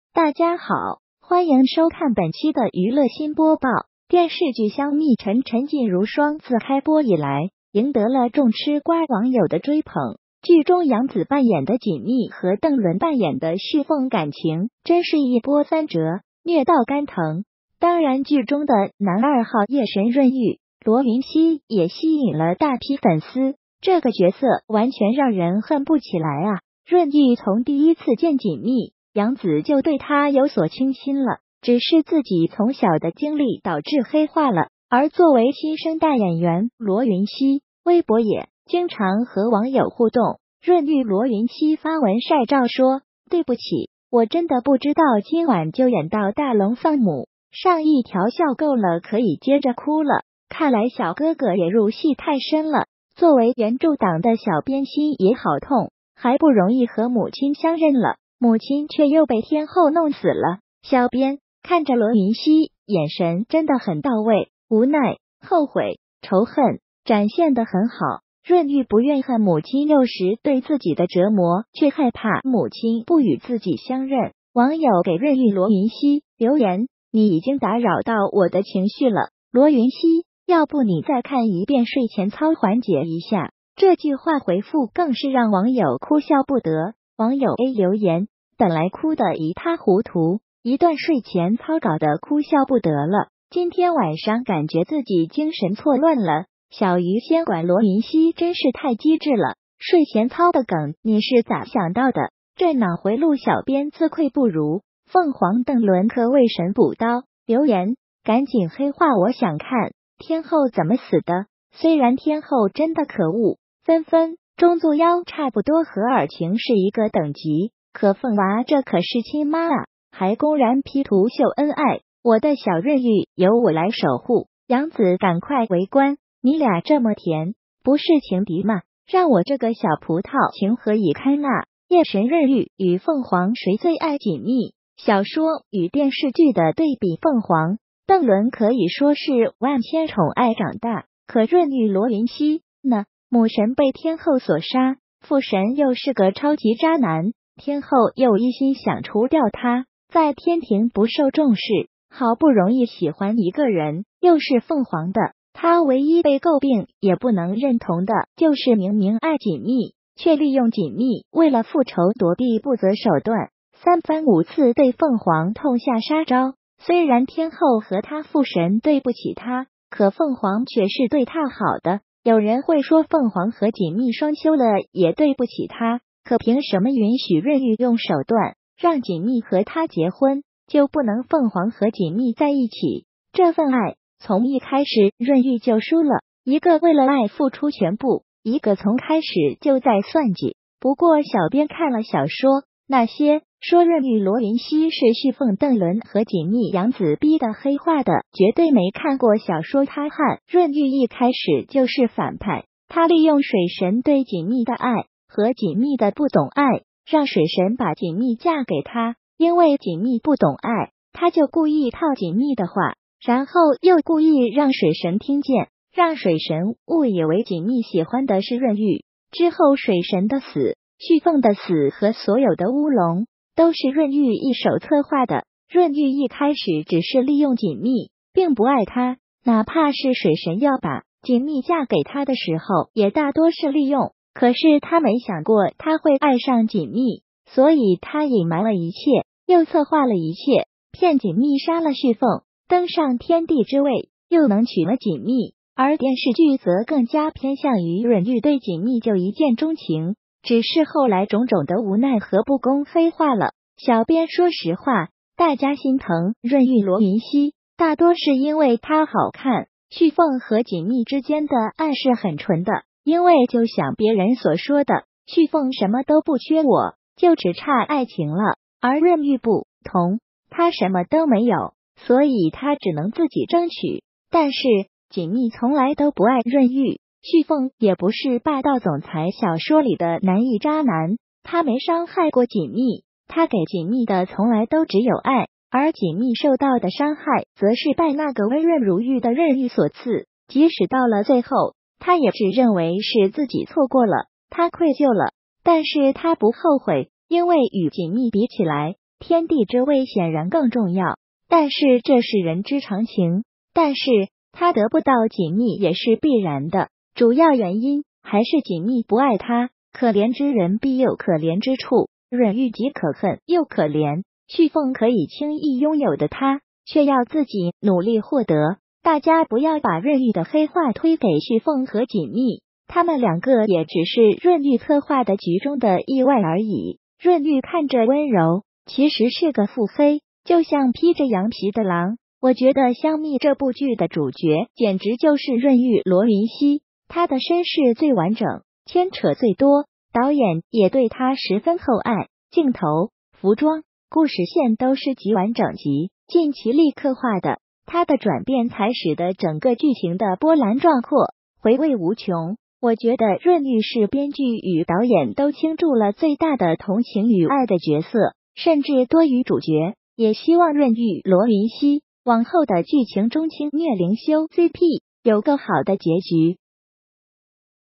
香蜜：润玉罗云熙入戏深，神回复让网友哭笑不得！邓伦：我守护你
大家好,欢迎收看本期的娱乐新播报. 电视剧《香蜜沉沉烬如霜》自开播以来，赢得了众吃瓜网友的追捧。剧中杨紫扮演的锦觅和邓伦扮演的旭凤感情真是一波三折，虐到肝疼。当然剧中的男二号夜神润玉（罗云熙）也吸引了大批粉丝，这个角色完全让人恨不起来啊！润玉从第一次见锦觅（杨紫）就对她有所倾心了，只是自己从小的经历导致黑化了，而作为新生代演员罗云熙，微博也经常和网友互动！润玉（罗云熙）发文晒照说：【对不起，我真的不知道今晚就演到大龙丧母，上一条笑够了可以接着哭了】看来小哥哥也入戏太深了。作为原著党的小编心也好痛，还不容易和母亲相认了，母亲却又被天后弄死了。小编，看着罗云熙眼神真的很到位，无奈、后悔、仇恨，展现的很好。润玉不怨恨母亲幼时对自己的折磨  却害怕母亲不与自己相认 。网友给润玉罗云熙留言：你已经打扰到我的情绪了！罗云熙：要不你再看一遍睡前操缓解一下？这句话回复更是让网友哭笑不得，网友A留言：【本来哭的一塌糊涂，一段睡前操搞得哭笑不得了，今天晚上感觉自己精神错乱了】小鱼仙馆罗云熙真是太机智了，睡前操的梗你是咋想到的，这脑回路小编自愧不如。凤凰（邓伦）可谓神补刀，留言：【赶紧黑化 我想看天后怎么死的】虽然天后真的可恶，分分钟作妖差不多和尔晴是一个等级，可凤娃这可是亲妈啊，还公然批图秀恩爱，我的小润玉由我来守护。杨紫赶快围观，你俩这么甜，不是情敌吗？让我这个小葡萄情何以堪那！夜神润玉与凤凰谁最爱锦觅？小说与电视剧的对比凤凰（邓伦）可以说是万千宠爱长大，可润玉（罗云熙）呢，母神被天后所杀，父神又是个超级渣男，天后又一心想除掉他，在天庭不受重视，好不容易喜欢一个人，又是凤凰的。他唯一被诟病也不能认同的就是明明爱锦觅却利用锦觅为了复仇夺帝不择手段，三番五次对凤凰痛下杀招。虽然天后和他父神对不起他，可凤凰却是对他好的，有人会说凤凰和锦觅双修了也对不起他，可凭什么允许润玉用手段让锦觅和他结婚，就不能凤凰和锦觅在一起。这份爱，从一开始润玉就输了，一个为了爱付出全部，一个从开始就在算计。不过小编看了小说，那些说润玉（罗云熙）是旭凤（邓伦）和锦觅（杨紫）逼的黑化的绝对没看过小说[擦汗]润玉一开始就是反派，他利用水神对锦觅的爱和锦觅的不懂爱让水神把锦觅嫁给他。因为锦觅不懂爱，他就故意套锦觅的话，然后又故意让水神听见，让水神误以为锦觅喜欢的是润玉。之后水神的死，旭凤的死和所有的乌龙都是润玉一手策划的 。润玉一开始只是利用锦觅，并不爱她，哪怕是水神要把锦觅嫁给他的时候也大多是利用。可是他没想过他会爱上锦觅，所以他隐瞒了一切，又策划了一切，骗锦觅杀了旭凤，登上天帝之位又能娶了锦觅。而电视剧则更加偏向于润玉对锦觅就一见钟情，只是后来种种的无奈和不公黑化了。小编说实话，大家心疼润玉罗云熙，大多是因为他好看。旭凤和锦觅之间的爱是很纯的，因为就想别人所说的，旭凤什么都不缺我，就只差爱情了。而润玉不同，他什么都没有，所以他只能自己争取。但是，锦觅从来都不爱润玉，旭凤也不是霸道总裁小说里的男一渣男，他没伤害过锦觅，他给锦觅的从来都只有爱。而锦觅受到的伤害则是拜那个温润如玉的润玉所赐。即使到了最后，他也只认为是自己错过了，他愧疚了，但是他不后悔，因为与锦觅比起来，天帝之位显然更重要，但是这是人之常情。但是，他得不到锦觅也是必然的，主要原因还是锦觅不爱他。可怜之人必有可怜之处，润玉即可恨又可怜，旭凤可以轻易拥有的他却要自己努力获得。大家不要把润玉的黑化推给旭凤和锦觅，他们两个也只是润玉策划的局中的意外而已。润玉看着温柔，其实是个腹黑，就像披着羊皮的狼。我觉得《香蜜》这部剧的主角简直就是润玉（罗云熙），他的身世最完整，牵扯最多。导演也对他十分厚爱，镜头，服装，故事线都是极完整极尽其力刻画的。他的转变才使得整个剧情的波澜壮阔，回味无穷。我觉得润玉是编剧与导演都倾注了最大的同情与爱的角色，甚至多于主角。也希望润玉（罗云熙），往后的剧情中轻虐灵修cp,有个好的结局！